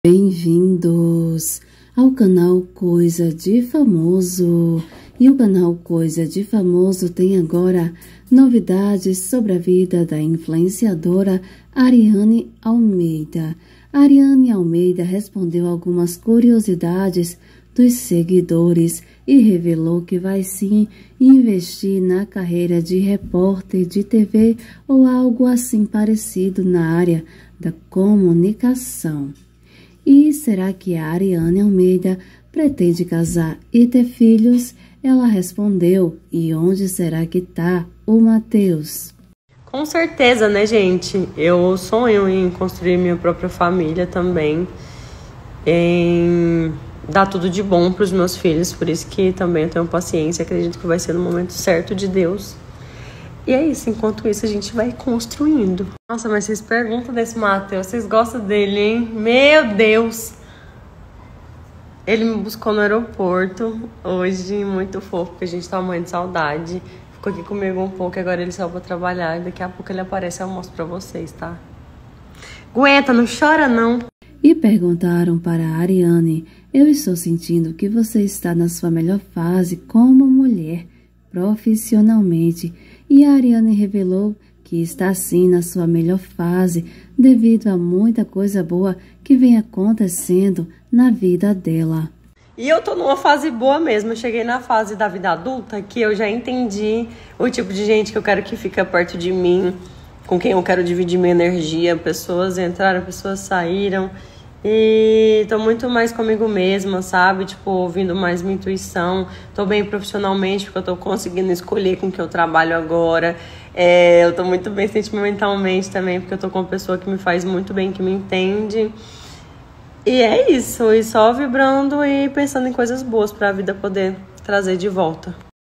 Hariany contou se vai casar e ter filhos + A melhor fase da sua vida.
COISA DE FAMOSO ❤️

O CANAL DAS MAIORES INFORMAÇÕES SOBRE OS FAMOSOS.

HARIANY ALMEIDA RESPONDEU SE PRETENDE CASAR E TER  FILHOS, ELA CONTOU QUE PRETENDE INVESTIR NA CARREIRA DE COMUNICAÇÃO.

CONTEÚDO  DA INFLUENCER
HARIANY ALMEIDA